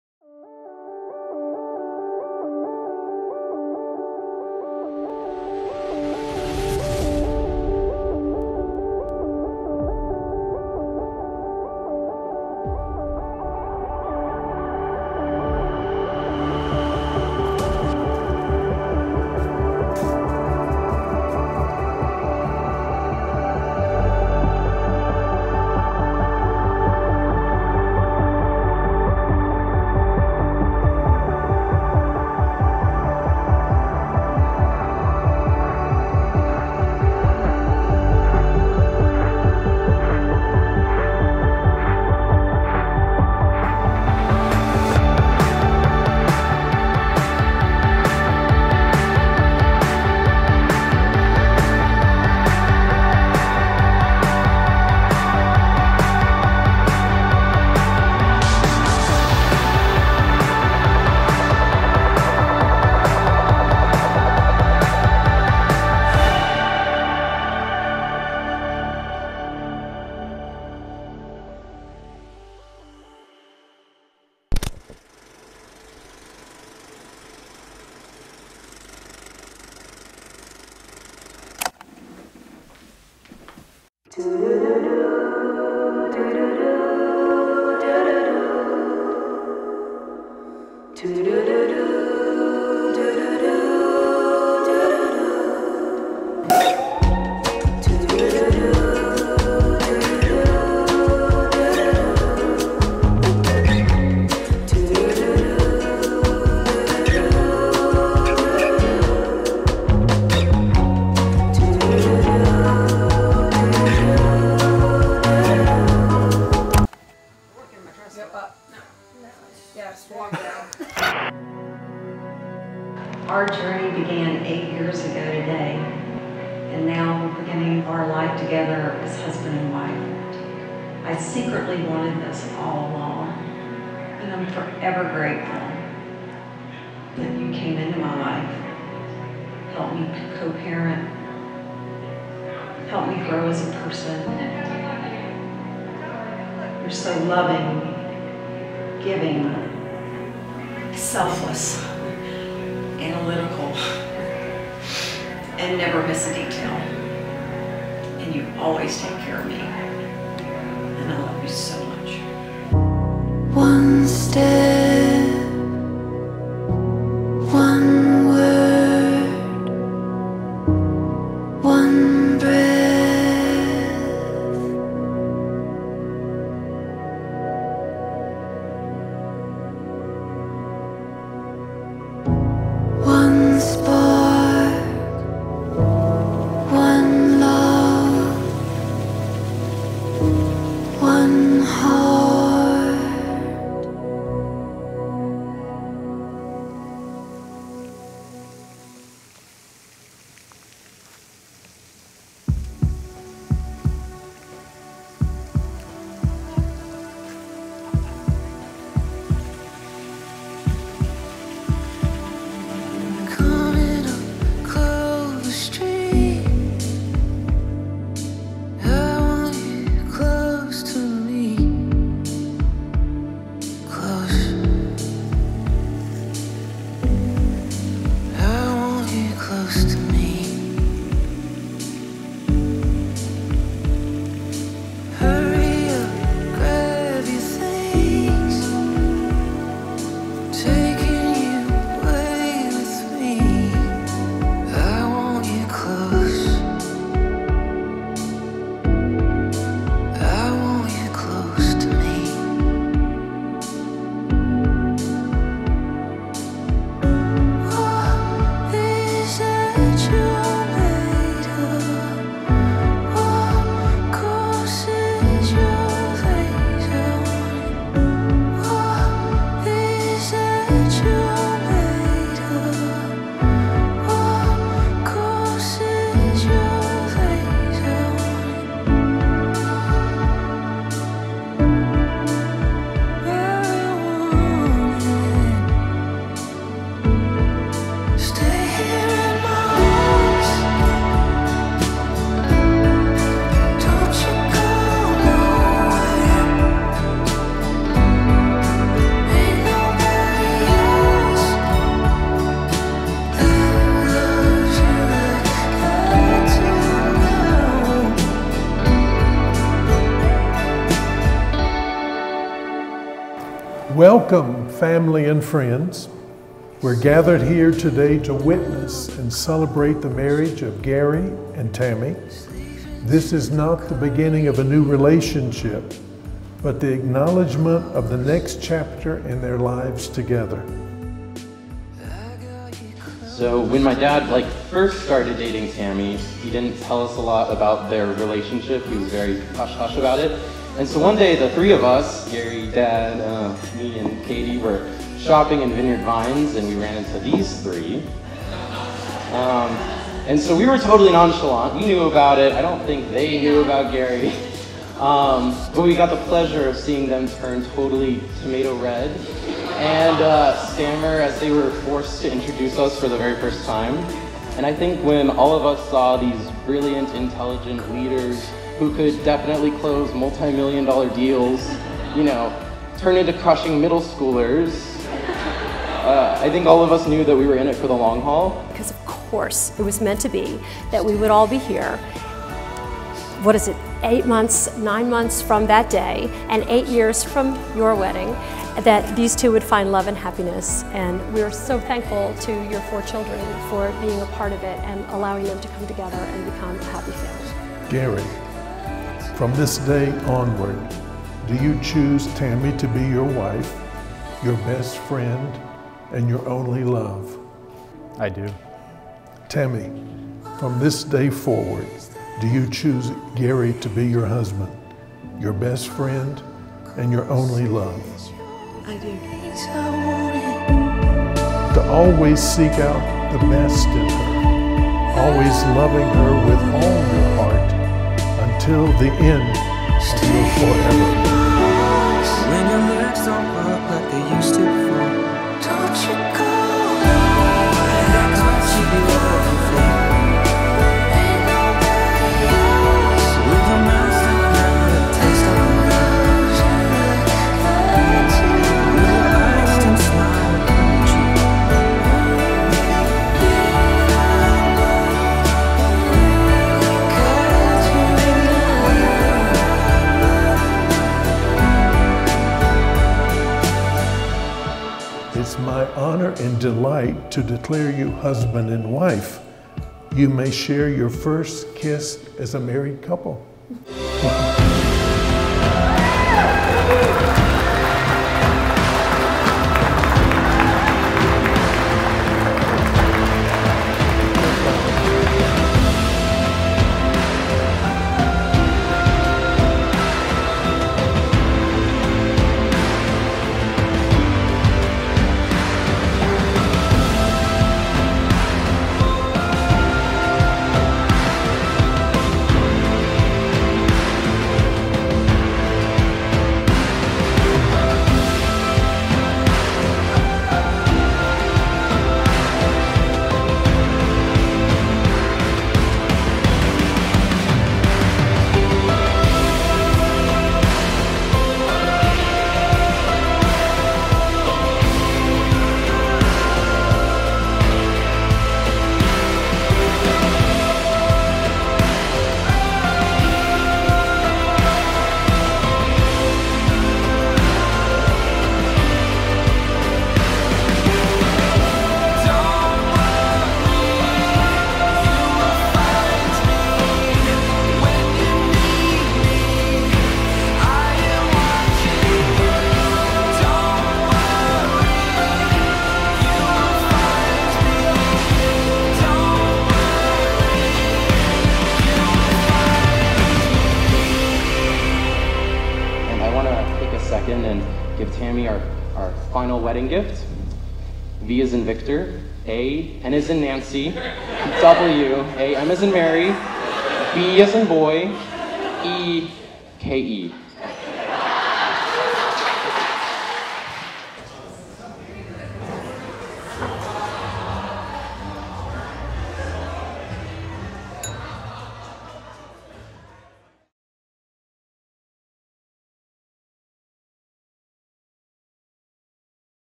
Tammy & Gary | The Old Edwards Inn Wedding | Highlands, NC
The wedding at Edwards Inn
Produced by Yours Always Films